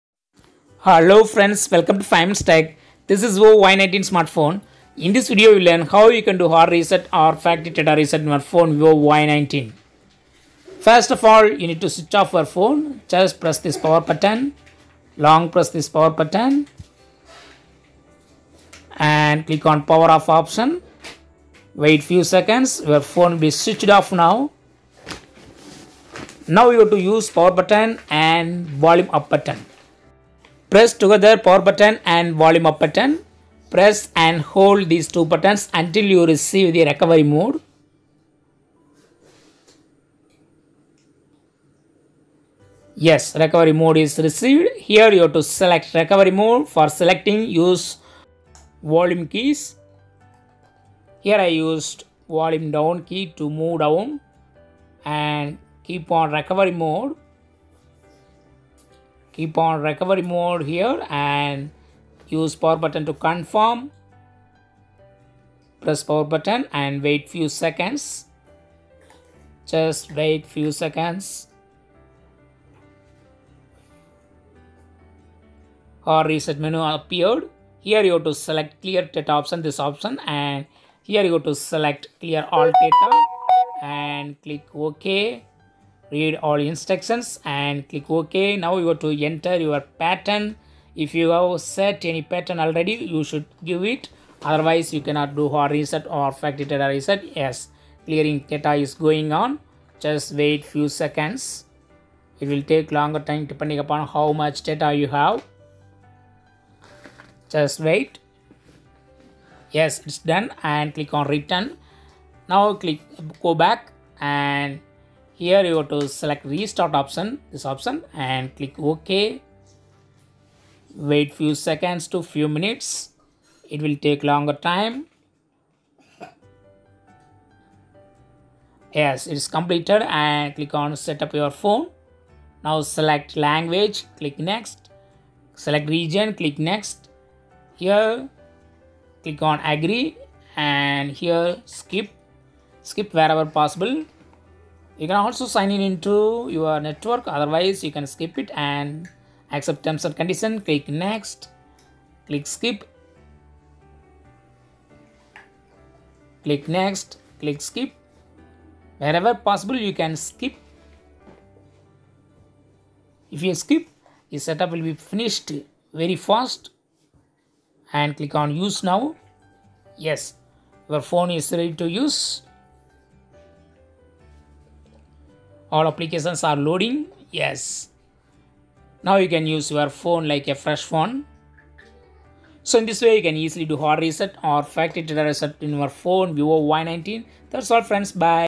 How to hard reset Vivo Y19
Looking for how to hard reset or factory data reset in your Vivo Y19? Here is the solution to your query. Hard reset can be done for many reason. One of the reason is that suppose if you sell your phone to another person and you don't want to keep your data in the phone and so you need probably hard reset or factory data reset. Another important thing is when your Y19 has a lot struggle like slow response, some malware attack, etc, you should then need hard reset or factory reset for your Vivo Y19. Note that if you have forgotten your Patten lock, then hard reset or factory data reset can't help you here like other phones. In Vivo models, pattern unlock can be done using security questions which you have answered while setting up your pattern itself. For more other reasons, you can do hard resetting or factory data resetting in Vivo Y19.